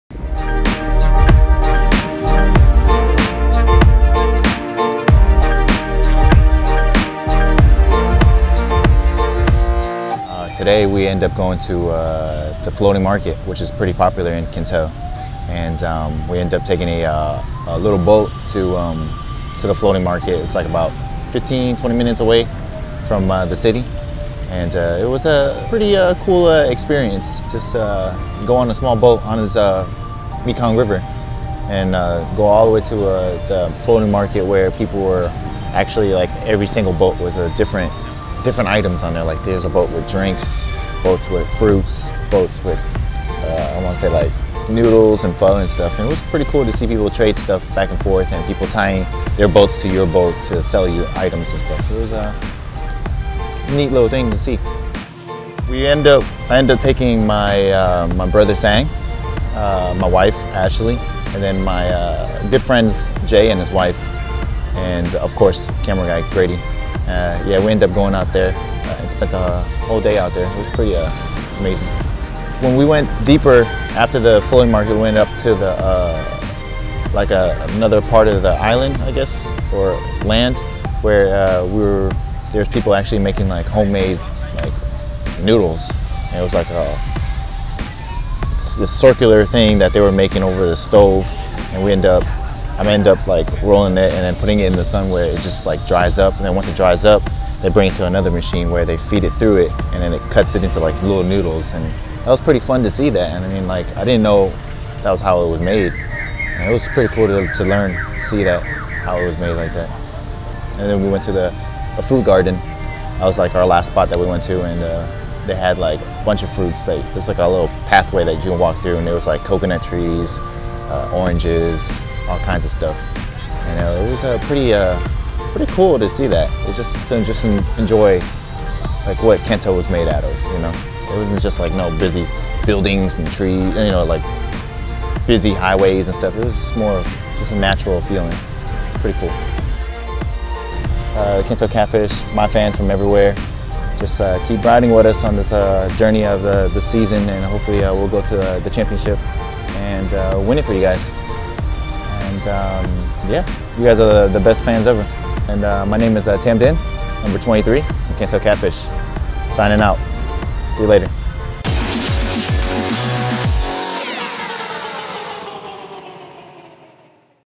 Tam Dinh exploring Can Tho Vietnam
Tam Dinh from Can Tho Catfish basketball club explores the Mekong Delta and the Floating Boat market.